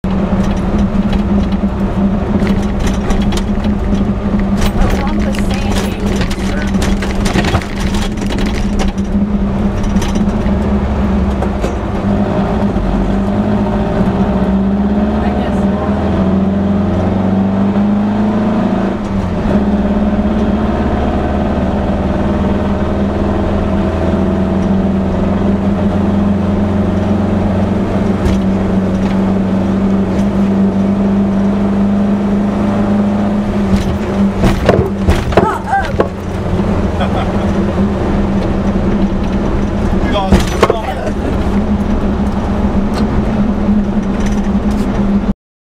little fast and bumpy.mpg
Padre Island in the Baja bug. Got a little bumpy.